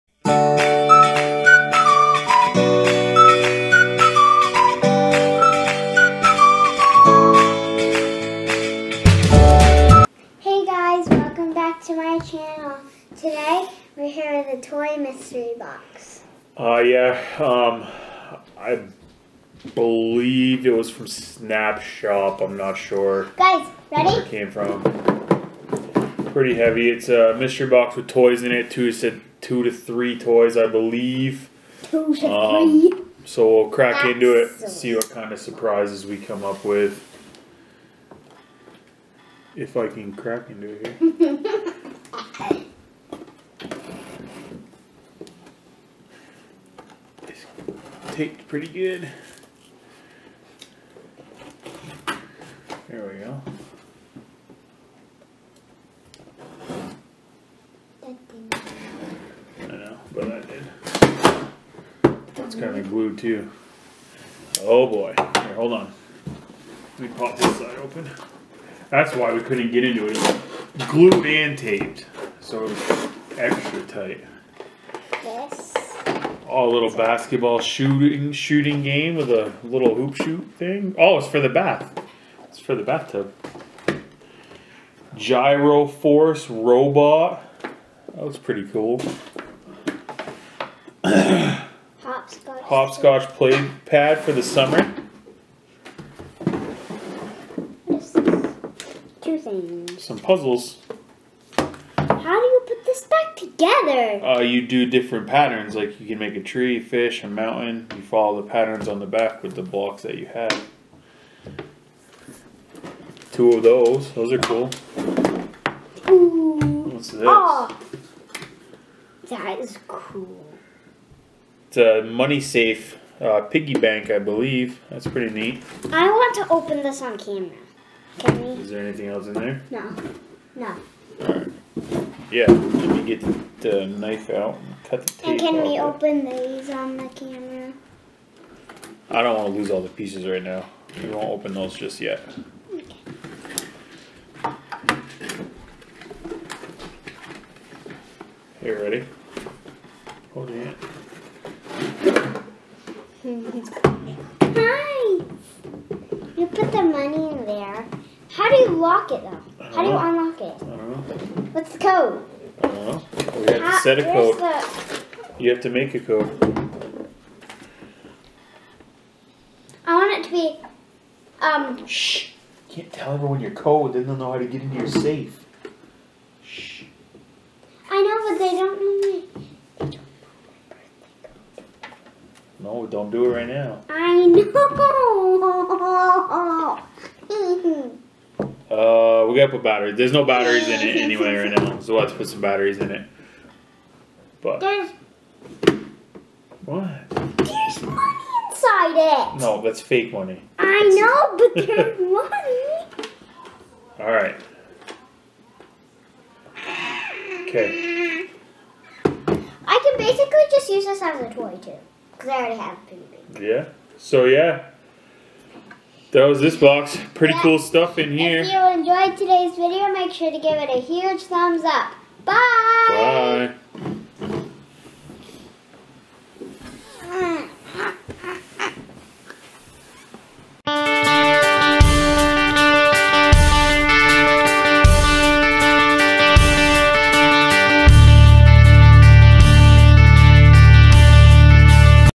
We opened a mystery box with some random toys in it!!
This box was cool, it had some toys for inside, and outside!!